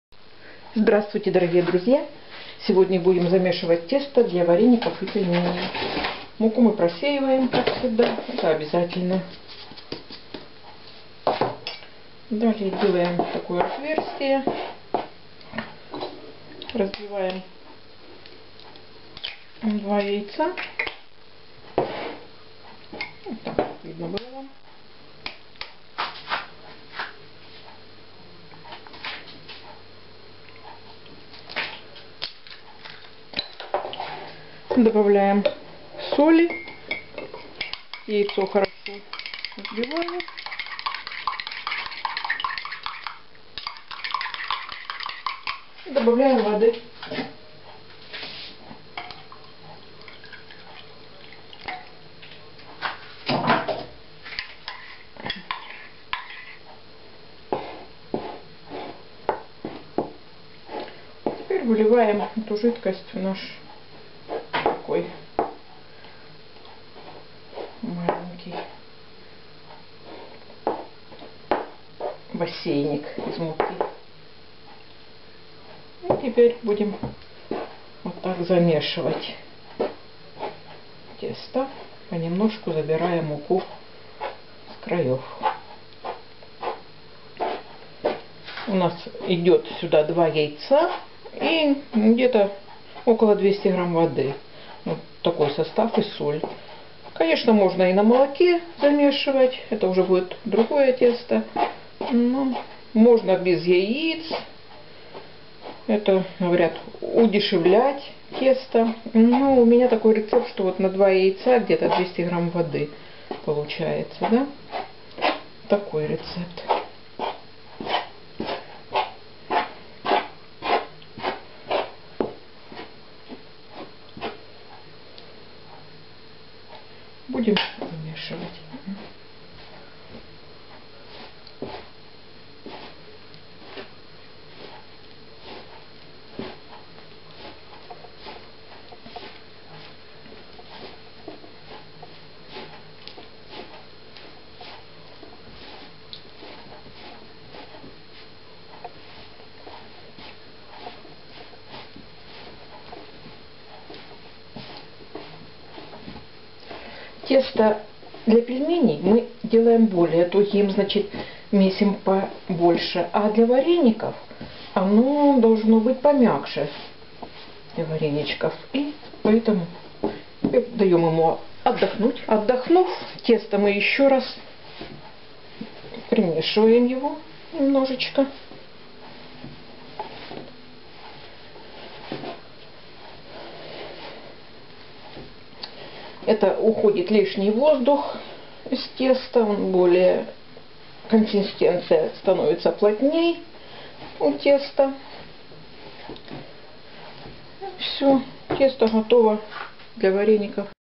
Тесто для вареников и пельменей
Для теста для вареников и пельменей нам  понадобиться 2 яйца , взбить, добавить теплой ( до 30 градусов) воды до 200 гр, посолить и замесить не тугое тесто. Тесто можно замесить и без яиц и оно будет очень хорошее.
Муки для теста 400 гр